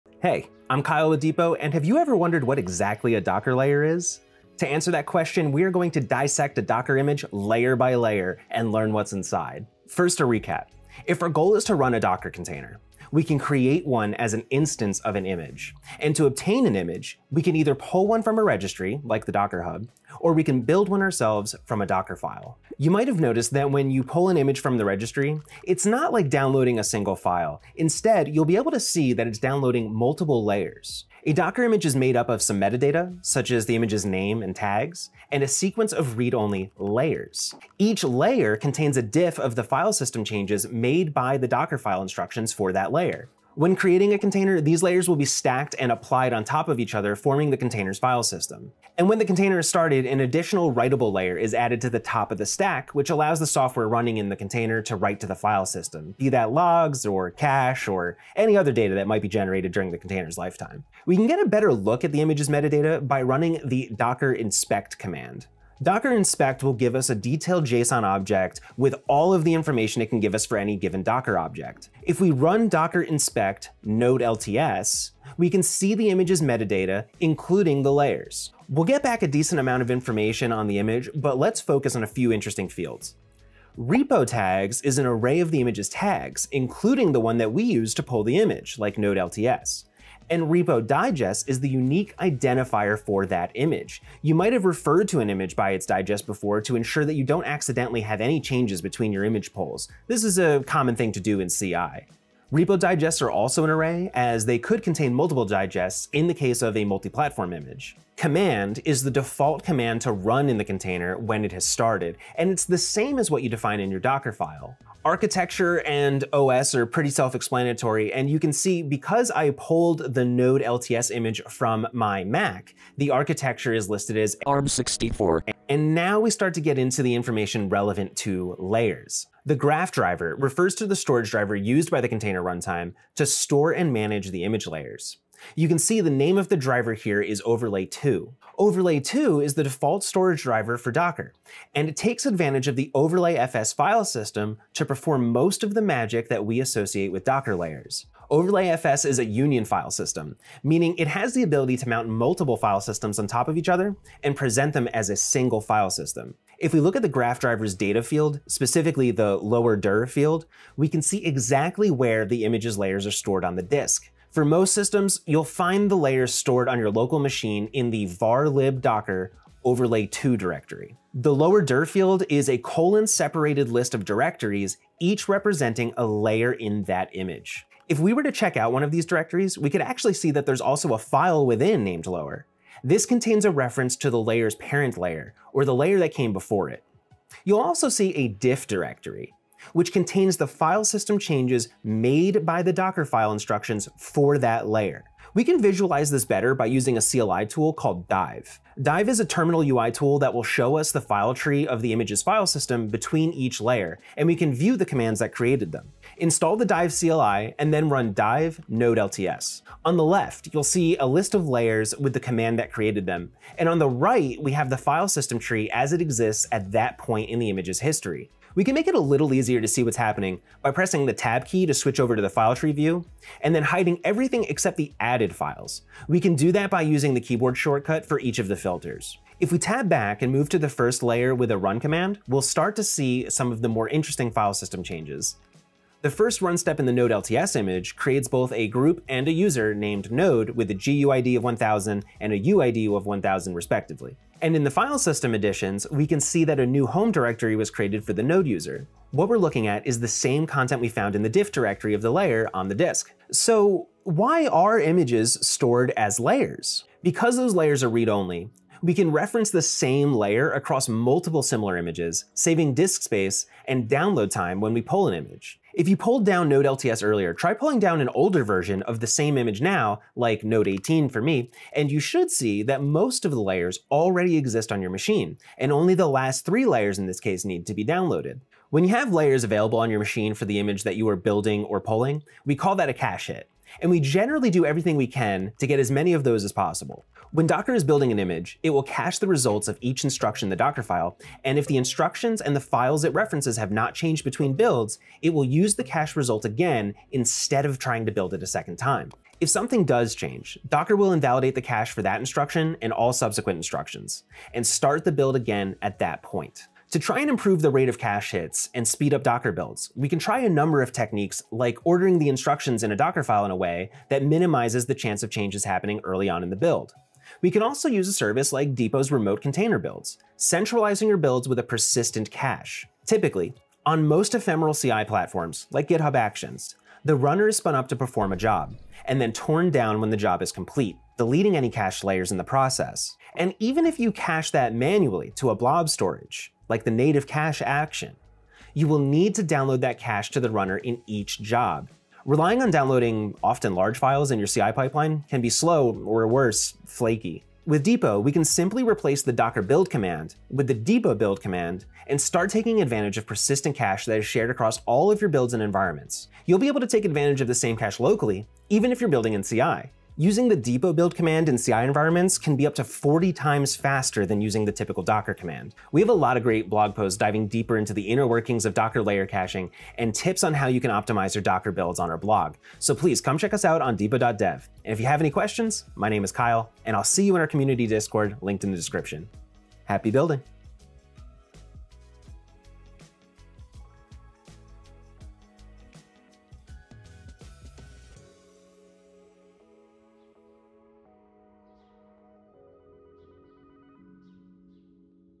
What Are Docker Layers Anyway?
Have you ever wondered what exactly a Docker layer is under the hood? Let's take a closer look and dissect a Docker image, layer by layer, to find out what it's made of.